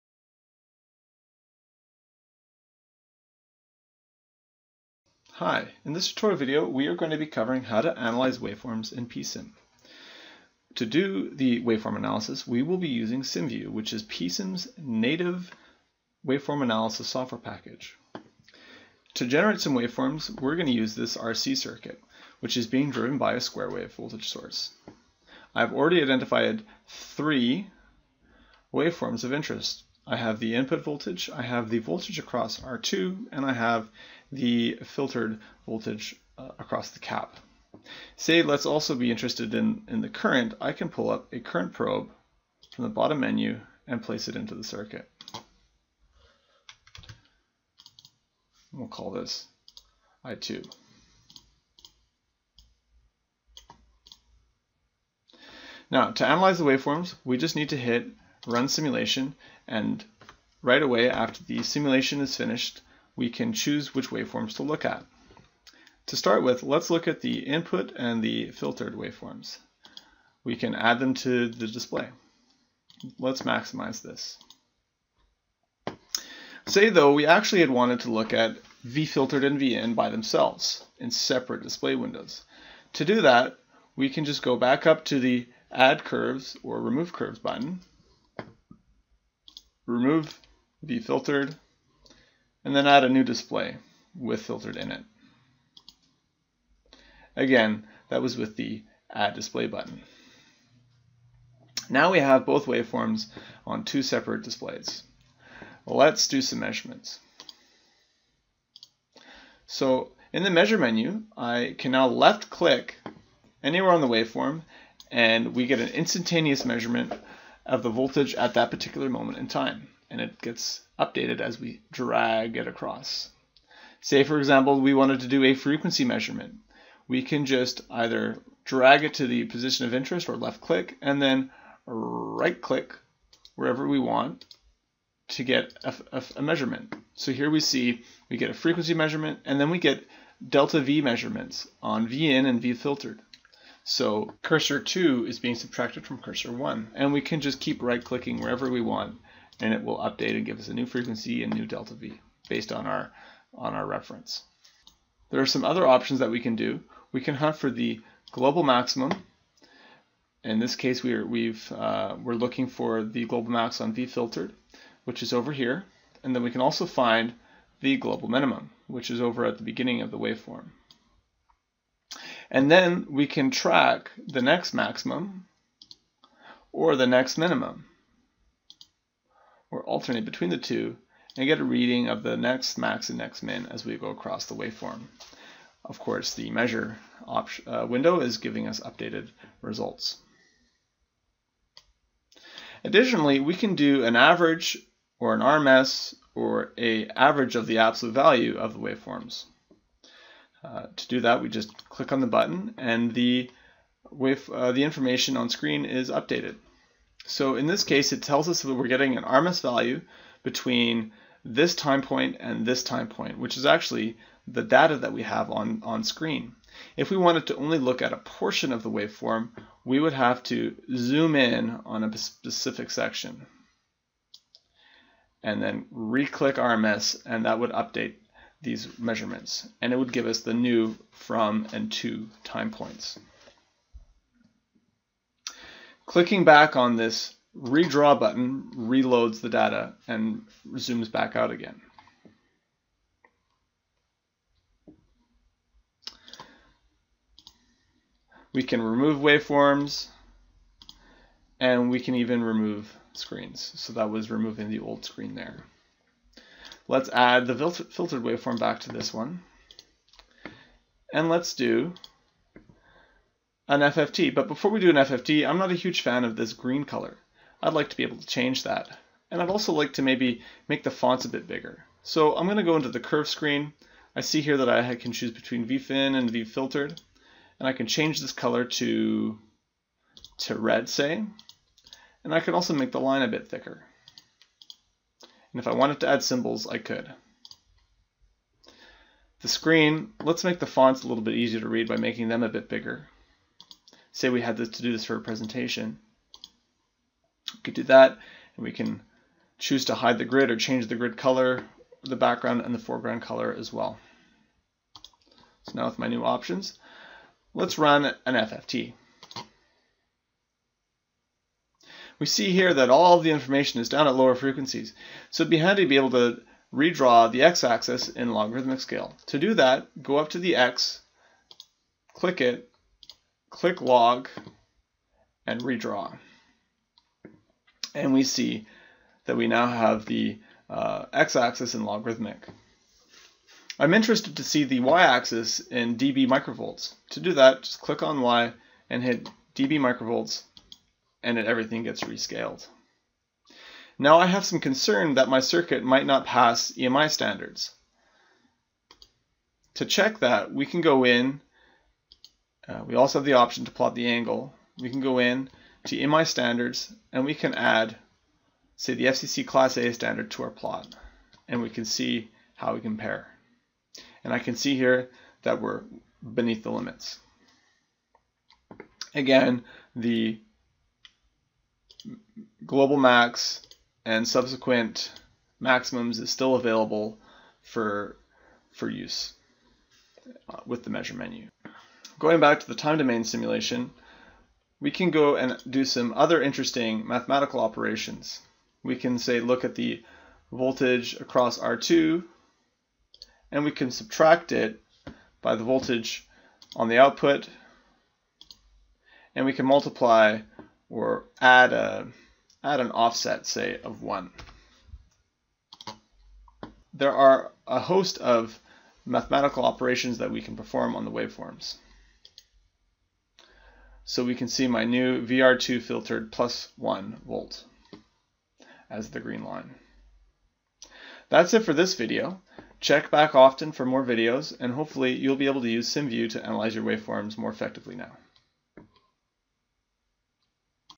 How to analyze waveforms with PSIM - v11 and older
This tutorial video covers how to analyze waveform data from PSIM simulations

Why is PSIM preferred for Power Electronics simulation?